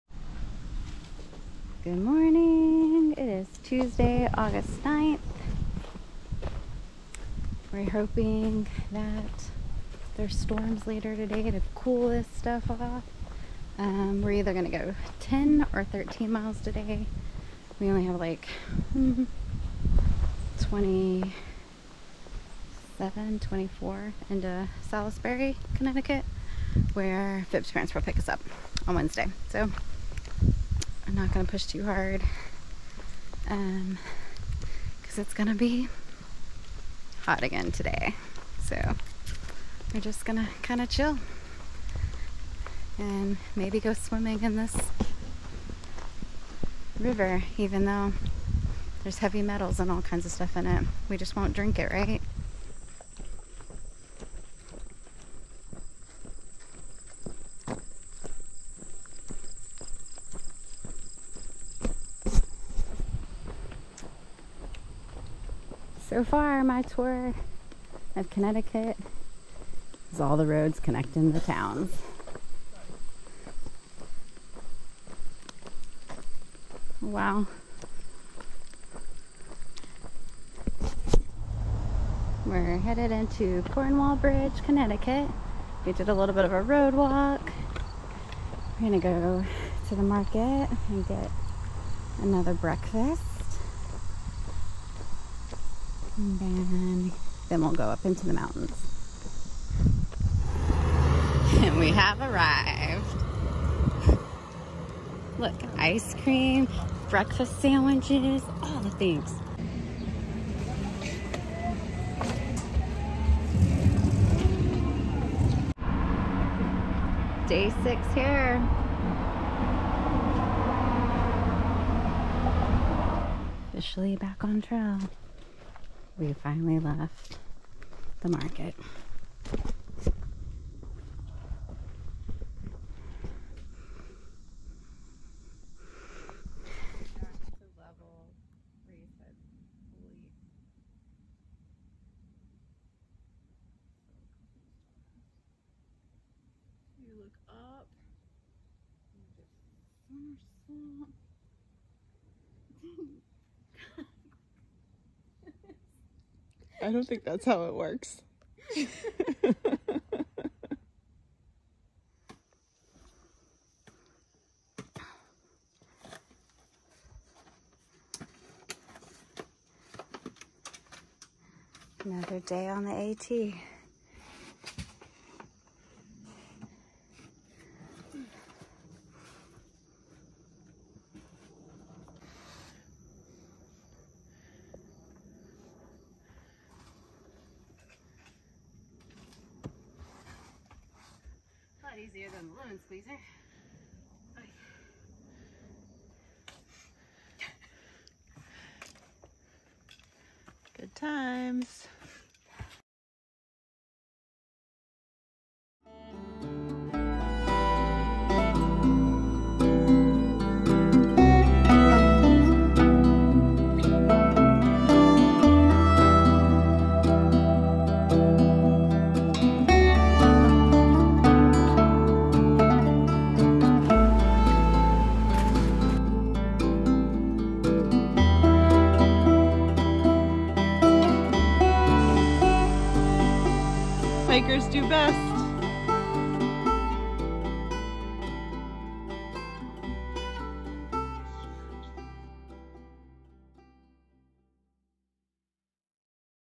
Day 139 | Day 2 of vertigo? | Cornwall Country Market | Appalachian Trail Thru Hike 2022
Day 139: Tuesday, August 9th
Stewart Hollow Brook Shelter to Pine Swamp Brook Shelter
I woke up hoping the vertigo was gone but it was still hanging on like it had nothing else better to do. Luckily, I could do some more road walking to Cornwall. We arrived at the country store and were there for an entire music playlist of the station they had on. Hey, they had good food, a comfortable patio and the music was great. When we heard the song that was playing when we arrived start playing again, we figured it was time to start hiking.
Did you know the liquor stores are called package stores or  the “packy” in the NE? I didn’t but the one in Cornwall gives hikers a free a beer if they sign their log.  They have a rock outside the store that is called “hiker rock.” It’a where most hikers relax and crack open their beer.
The climb out of Cornwall was tough because my vertigo was getting worse. (I had not drank a beer at the packy.) We stopped at the top of the ascent and I googled exercises for vertigo. Fib got quite a kick out of my attempt at these exercises. 100% did not do them correctly.
We made it to the shelter and there were 10 other tents including fellow Texans: Taxi and Charger. I’ve met quite a few Texans lately. We are all at the back of the NOBO group…not sure what that means.
Miles hiked: 10
AT mile Marker: 1488.3